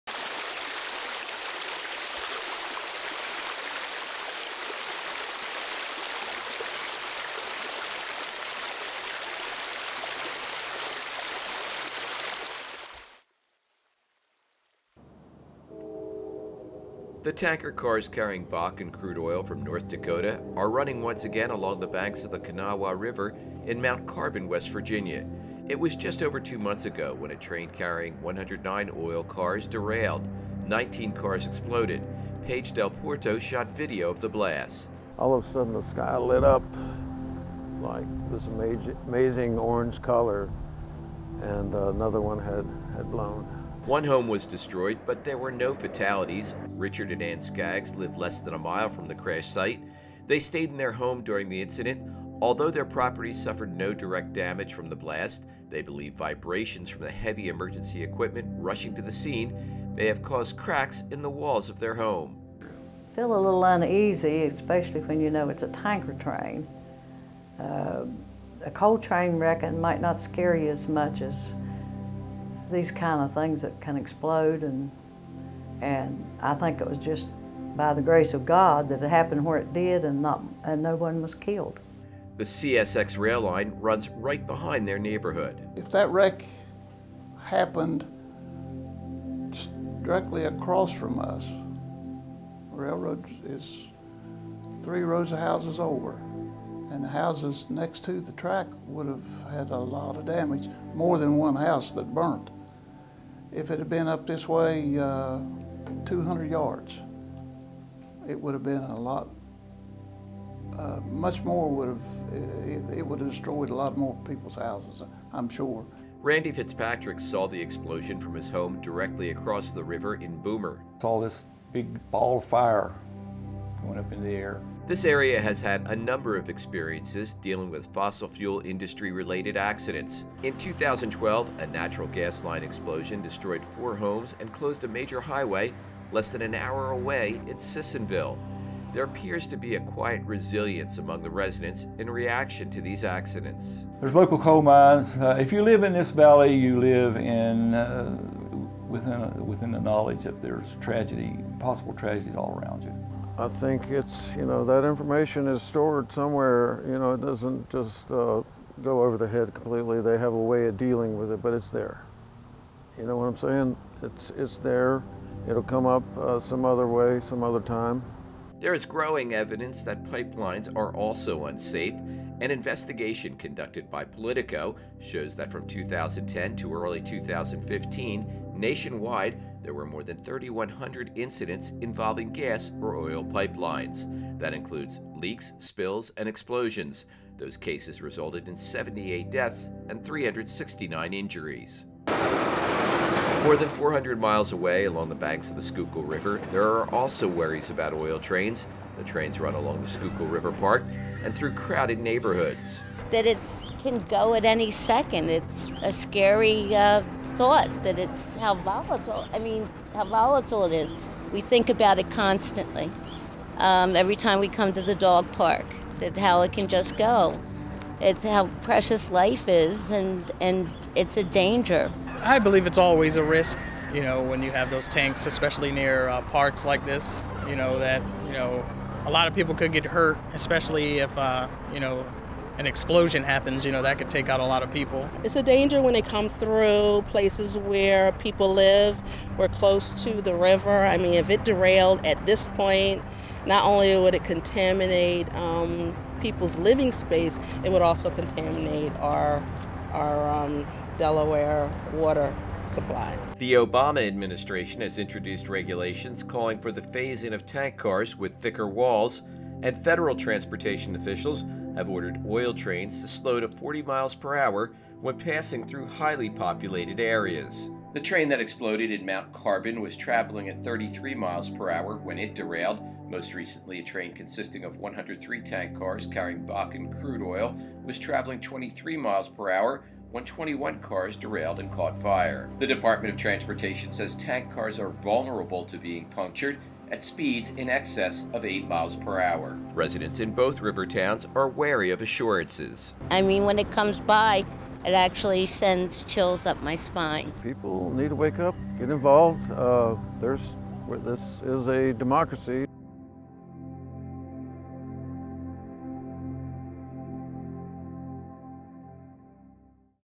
Delaware Riverkeeper Network video Oil Train experiences
Transporting fossil fuels is dangerous.    This video examines the experiences of one area of the country that has experienced an oil train derailment and fire as well as a gas transmission pipeline explosion.  Residents living in one Northeast city also discuss their worries about the threat from oil trains that pass through their neighborhoods.